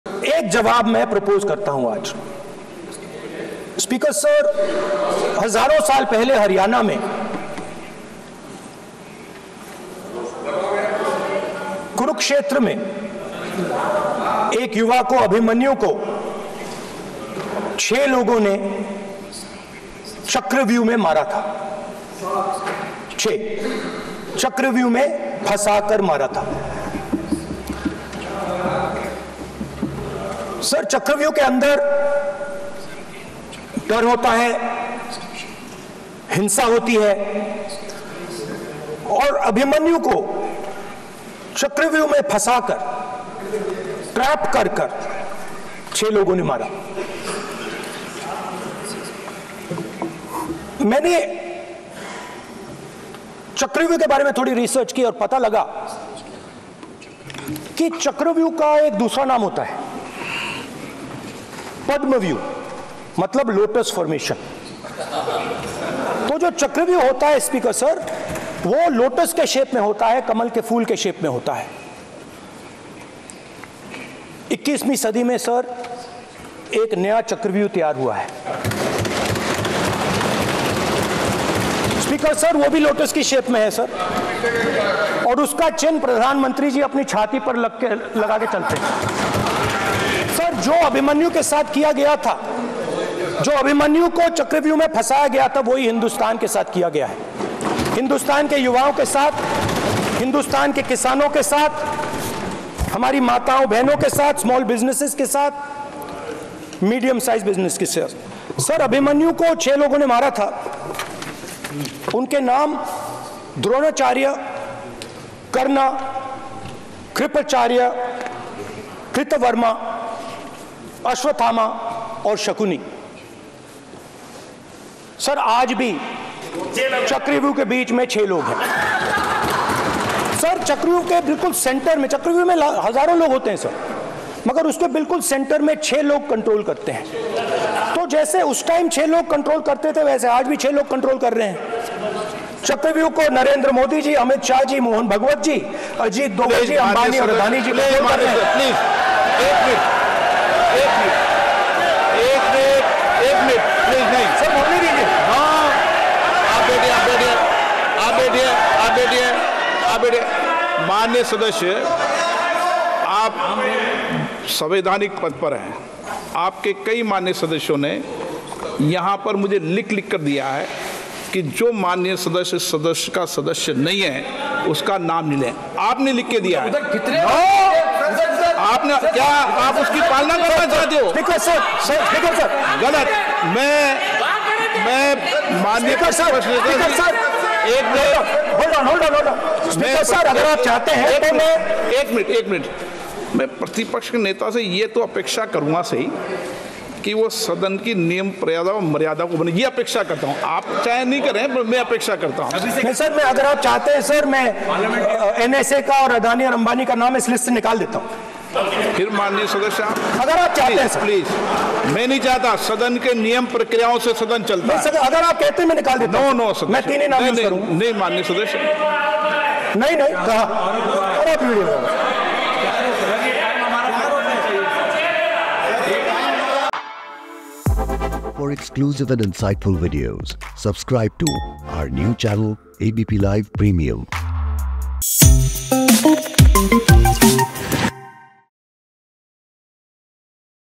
Parliament Session 2024: 'अभिमन्यु को फंसाकर छह लोगों ने मारा..' Rahul ने PM Modi पर बोला हमला
Rahul Gandhi In Lok Sabha: संसद के मानसून सत्र के दौरान सोमवार (29 जुलाई) को लोकसभा में विपक्ष के नेता राहुल गांधी केंद्र सरकार पर जमकर निशाना साधा. उन्होंने सदन में महाभारत की कथा का जिक्र करते हुए कहा कि भारतीय जनता पार्टी (बीजेपी) ने देश की जनता को चक्रव्यूह में फंसाया. कांग्रेस सांसद ने कहा कि जो हजारों साल पहले अभिमन्यु के साथ किया गया वो आज देश की जनता के साथ किया जा रहा है.

उन्होंने कहा, "हजारों साल पहले हरियाणा के कुरुक्षेत्र में अभिमन्यु को छह लोगों ने चक्रव्यूह में मारा था. इसके अंदर डर होता है, हिंसा होती है और अभिमन्यु को फंसाकर छह लोगों ने मारा. मैंने जब इसके बारे में रिसर्च की तो पता चला कि उसका एक और नाम होता है पद्म व्यूह. ये लोटस के शक्ल में होता है."